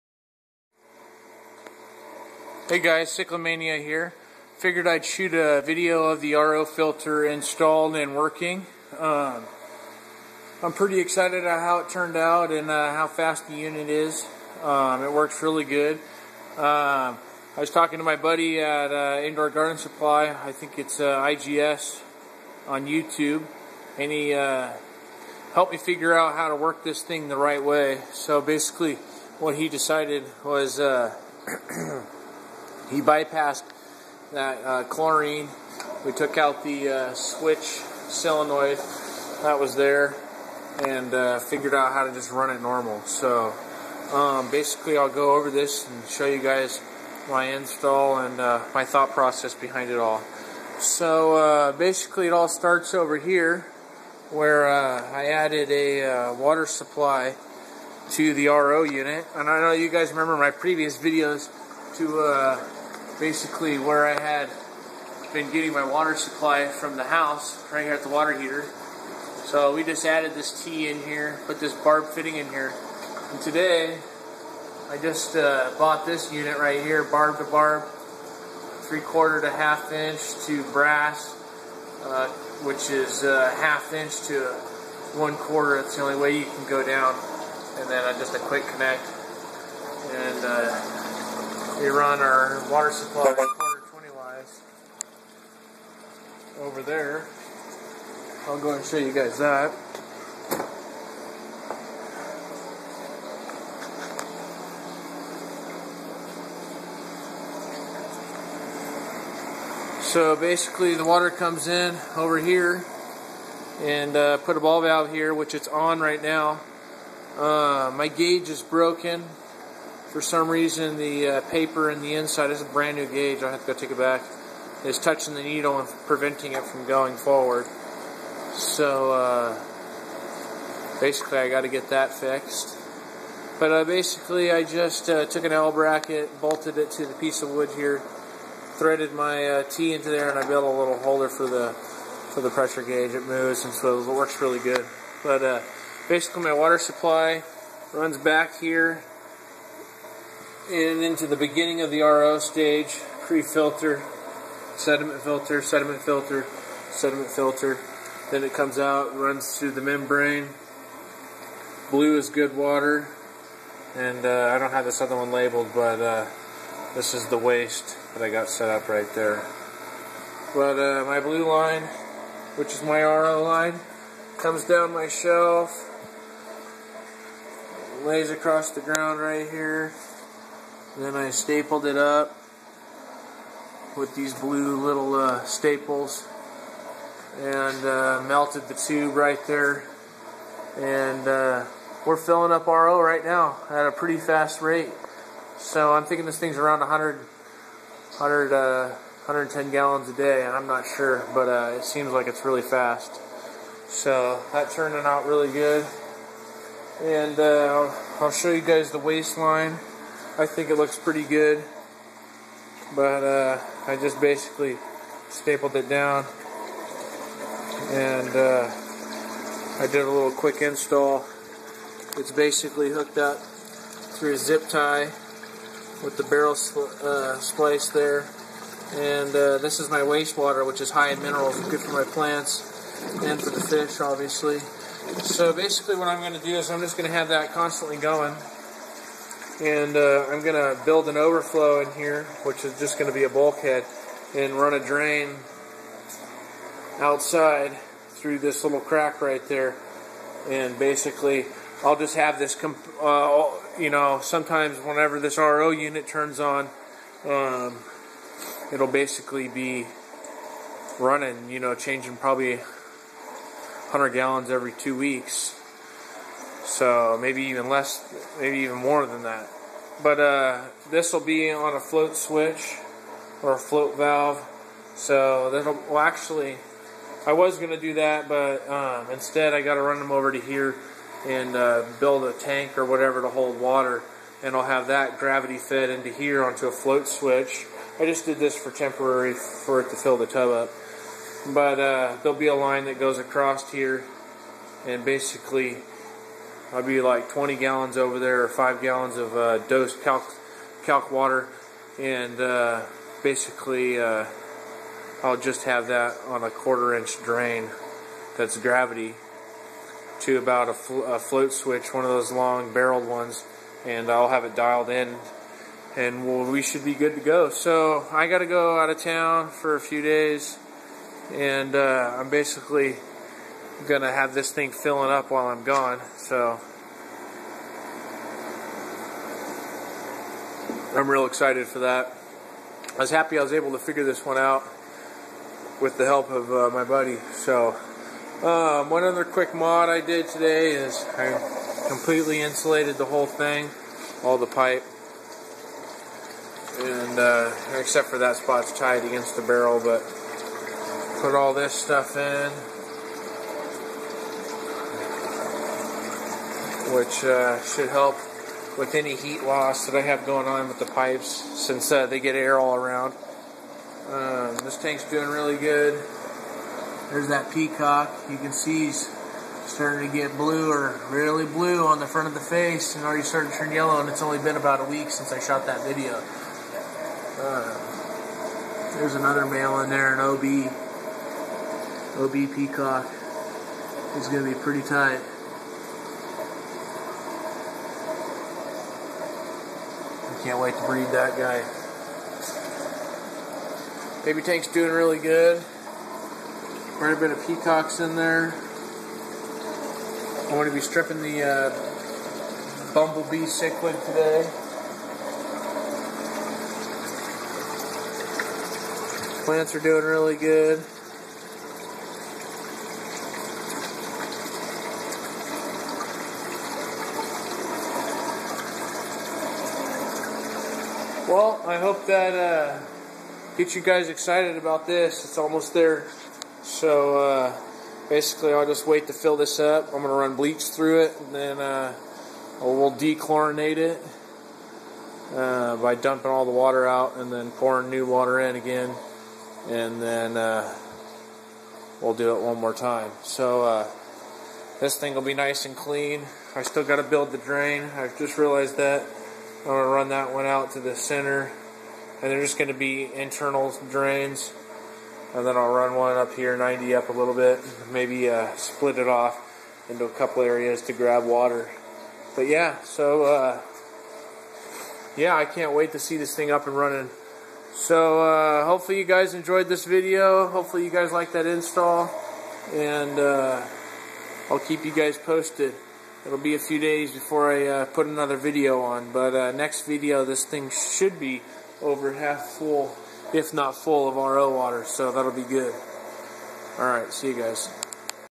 RO filter installed and running (fishroom updates)
Hey guys! I shot this video too show my newly installed ro filter. I hope you guys like the install. I think it turned out pretty clean. I also added insulation around the pvc pipes.